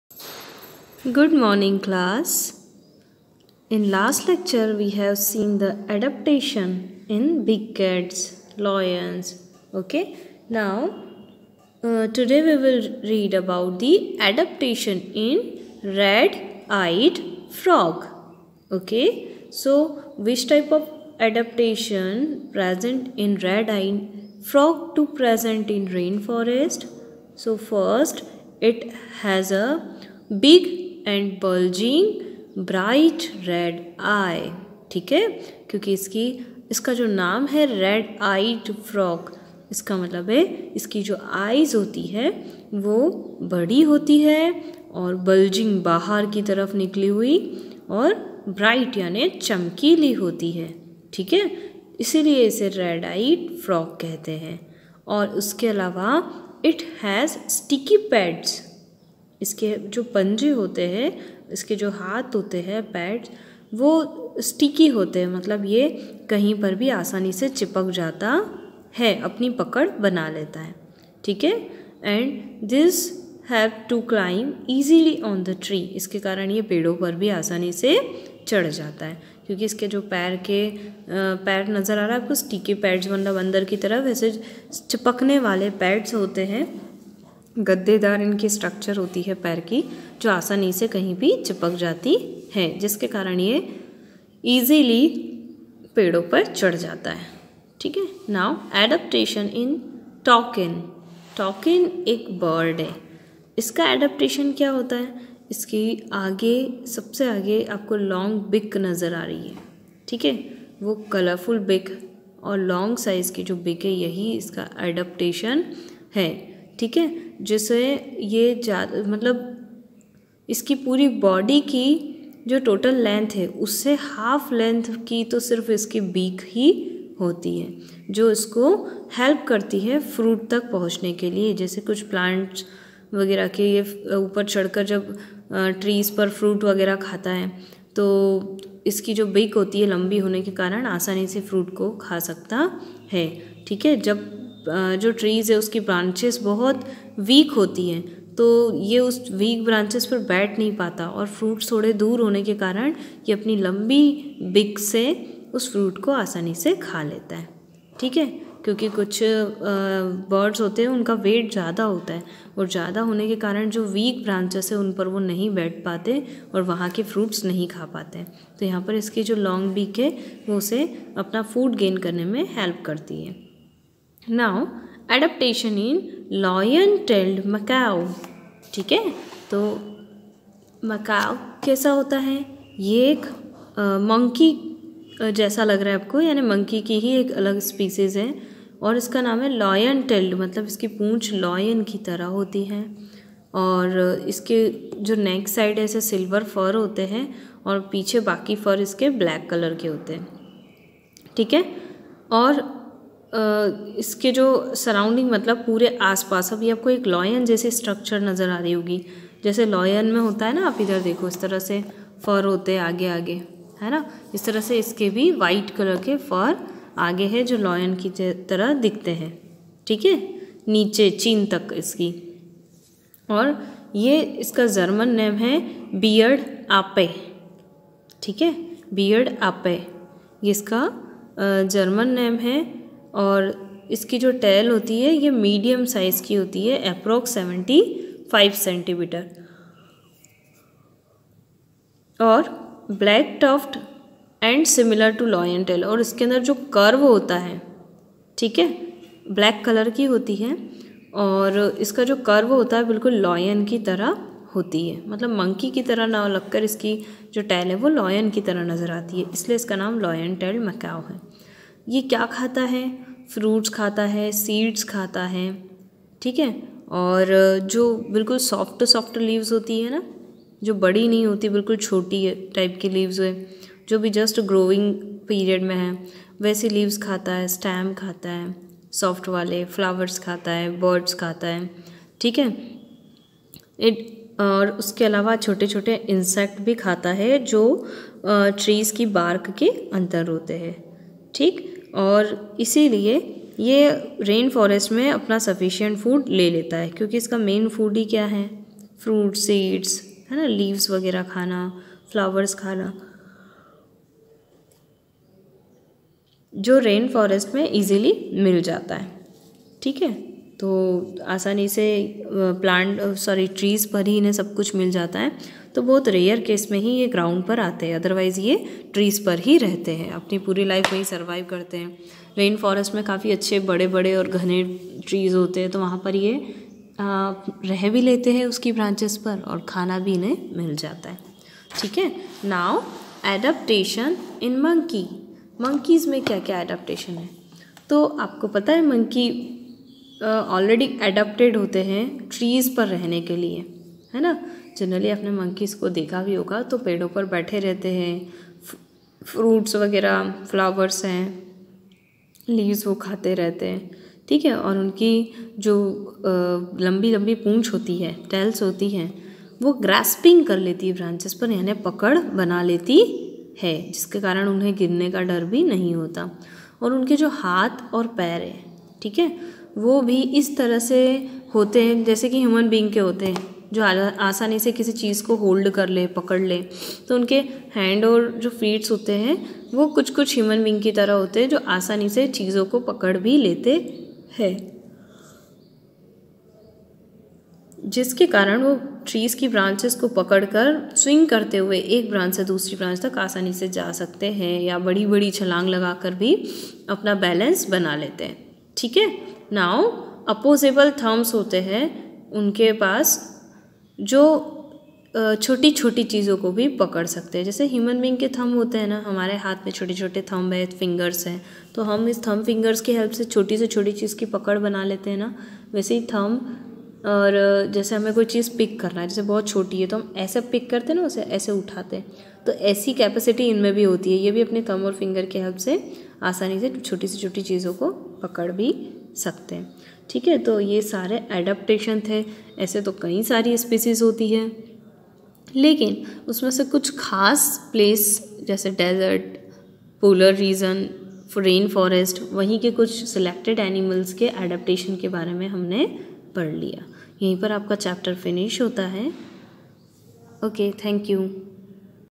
Class 7th Science Chapter 7 part 6
adaptation in red eyed frog,toucan and lion tailed macaque